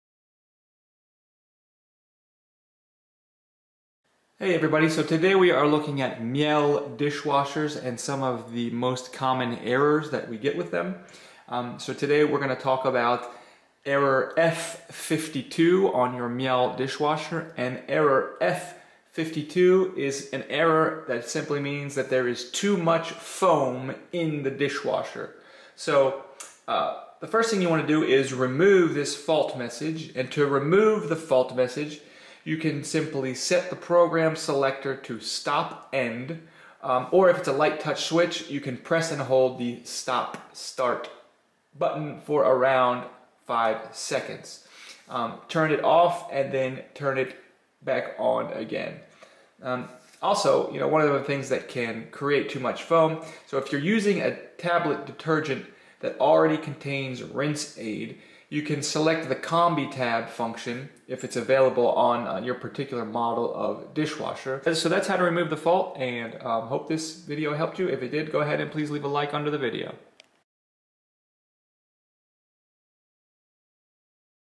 Error F52 Miele Dishwasher | How to Fix
If your Miele dishwasher is showing fault F52 this video explains how to fix and remove the error.